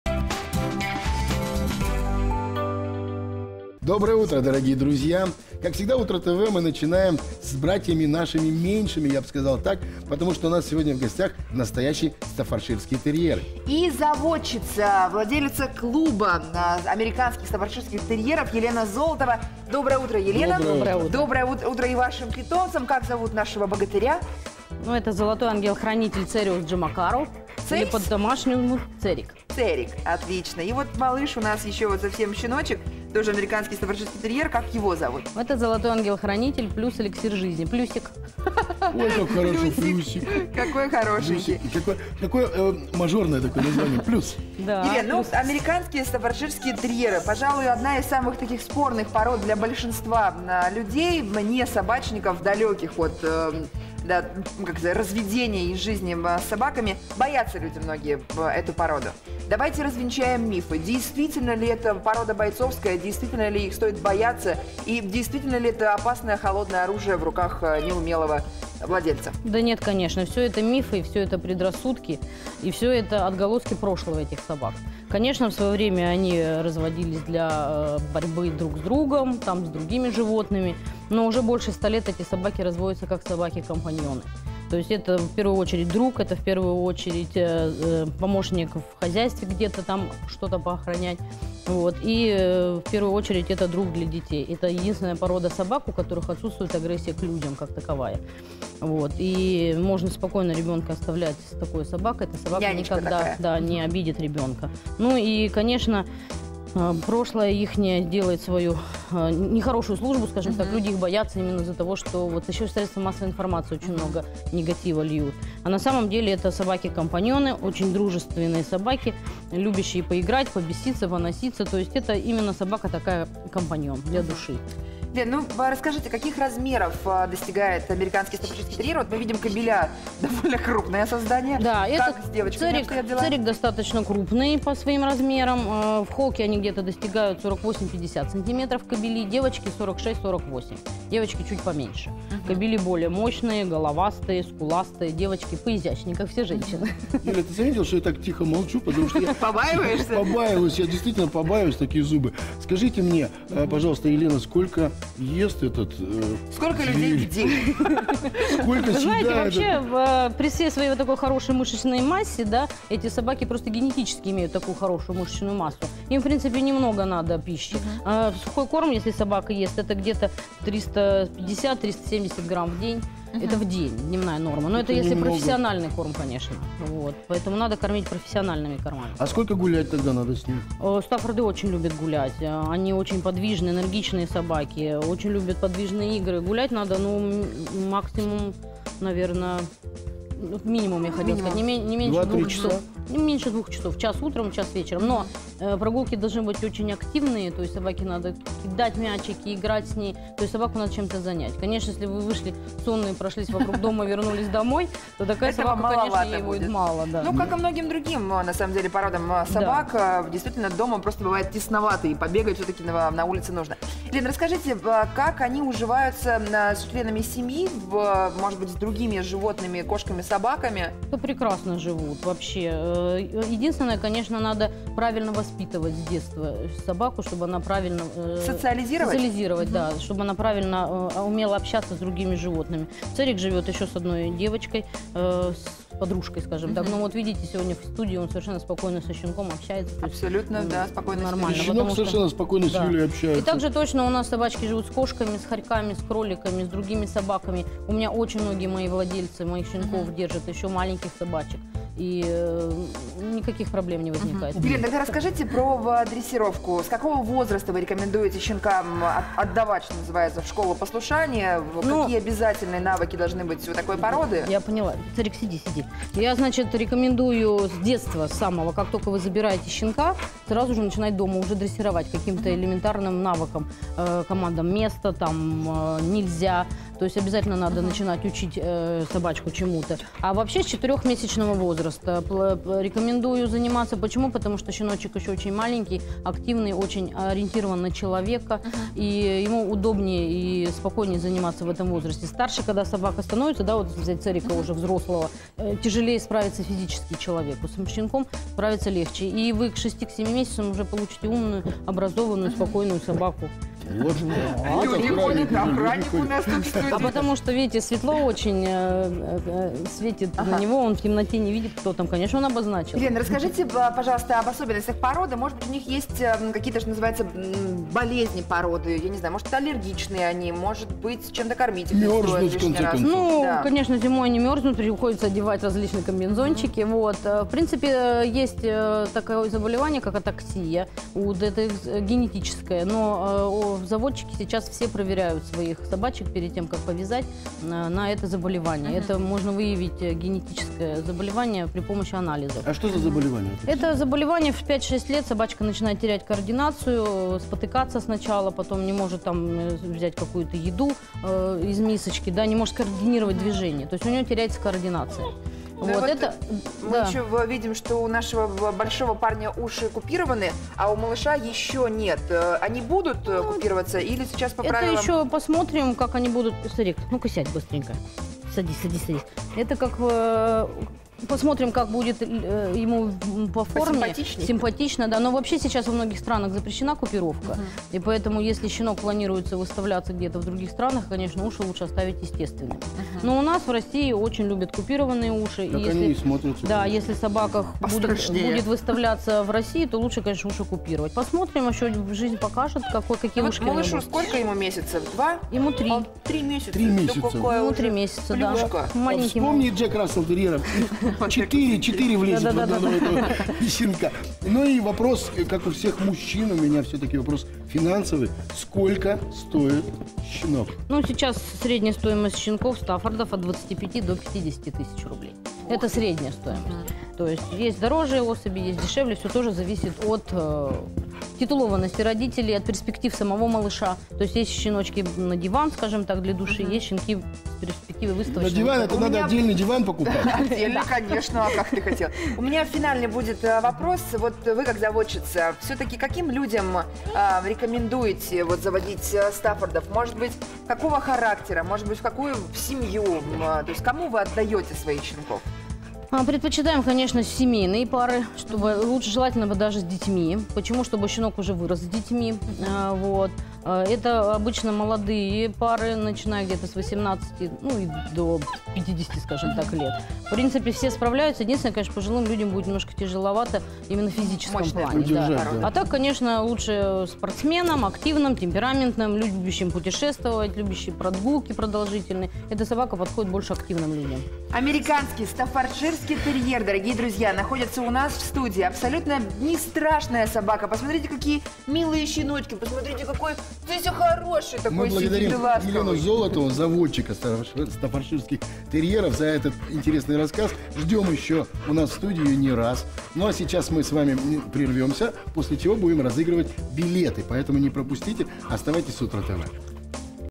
Знакомство с питомцами: стаффордширский терьер Церик (17.11.15)
И снова у нас в гостях красивые, умные и обаятельные собаки, на этот раз — стаффордширские терьеры Церик и Плюсик. Поддаётся ли эта порода дрессуре и как содержать в квартире охотников-стаффордов? Рассказывает владелица питомника Елена Золотова!